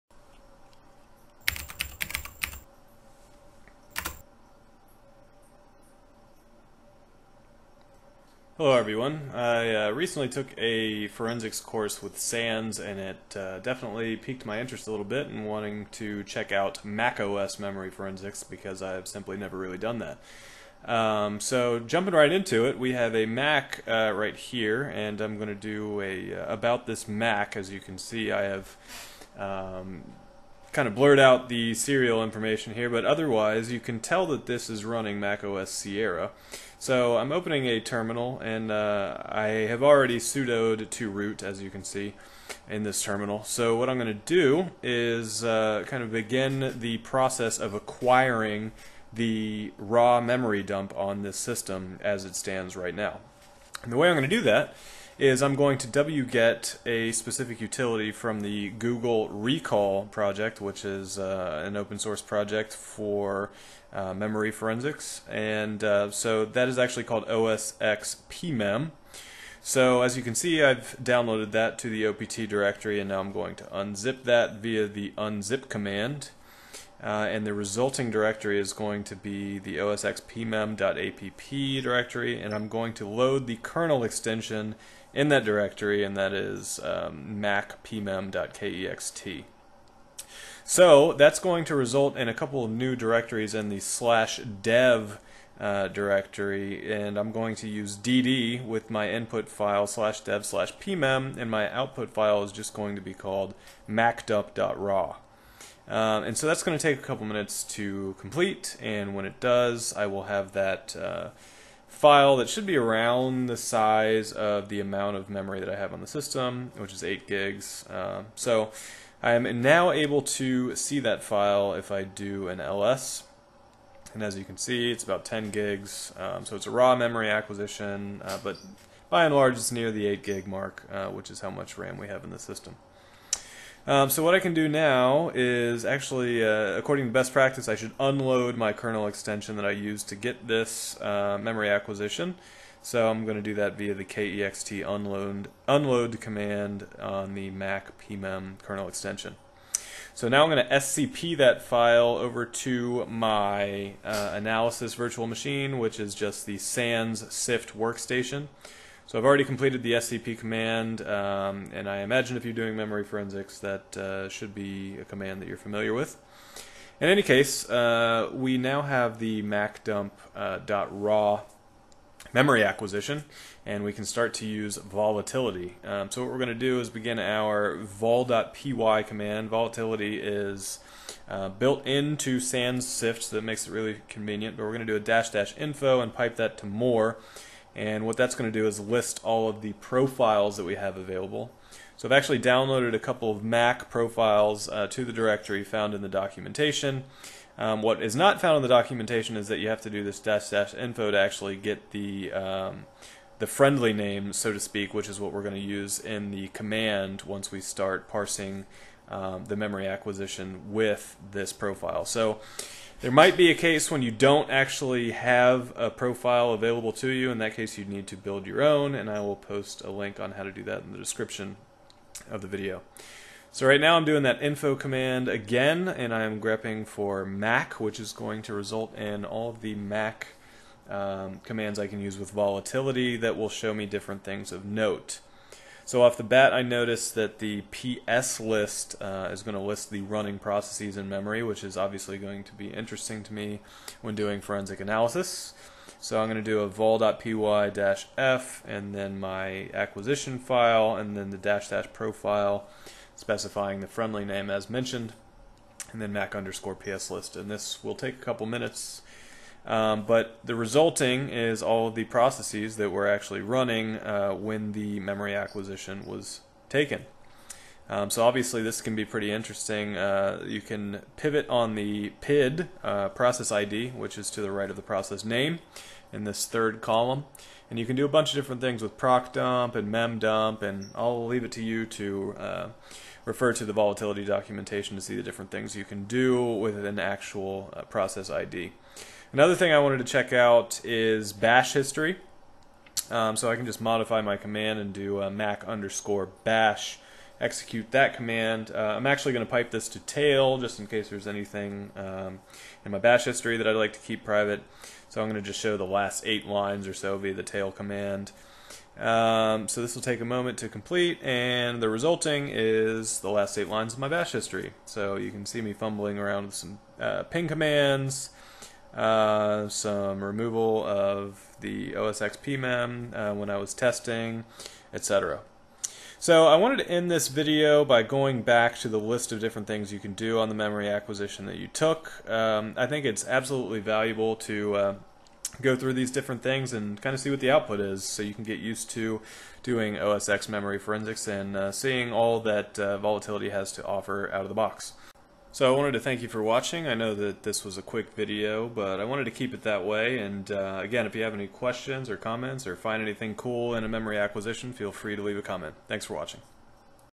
macOS Forensics - Memory Acquisition and Analysis in Volatility
After taking a SANS course I wanted to learn more about memory forensics, specifically in macOS.  I found the following blog post which helped in the creation of this video:

In the video I mention downloading Mac profiles which can be found on the volatility github here:

And lastly, if you (unfortunately) have to create your own profile, you can refer to the first blog I mentioned.